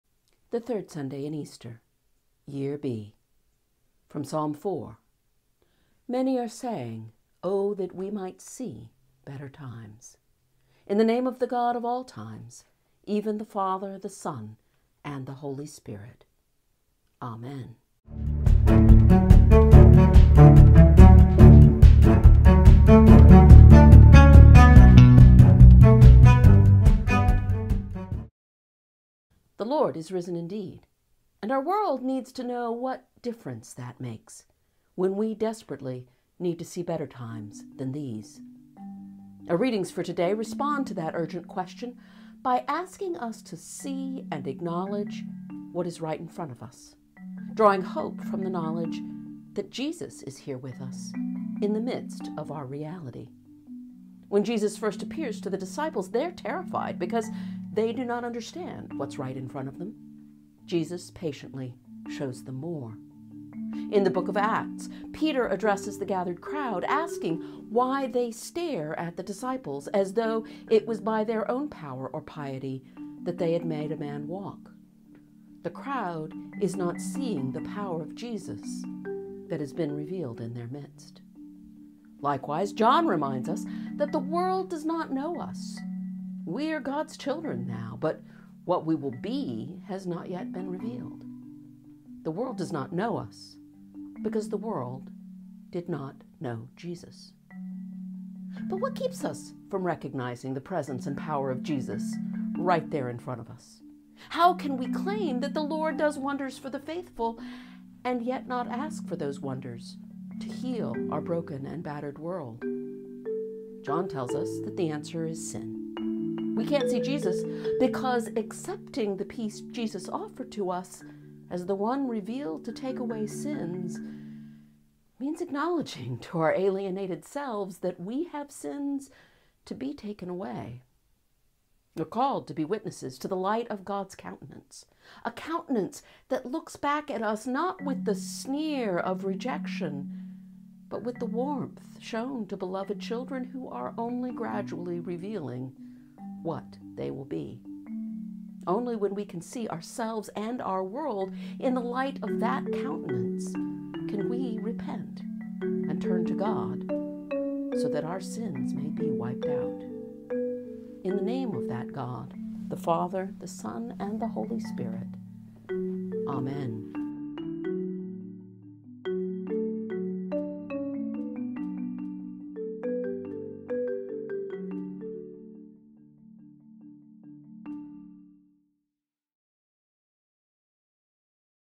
Better Times
A Sermon on the readings for the 3rd Sunday in Easter, Year B:

Acts 3:12-19
Psalm 4
1 John 3:1-7
Luke 24:36b-48

The full version of this sermon was preached as a part of Sunday Eucharist at Holy Trinity Episcopal Church in South Bend, Indiana on April 18, 2021.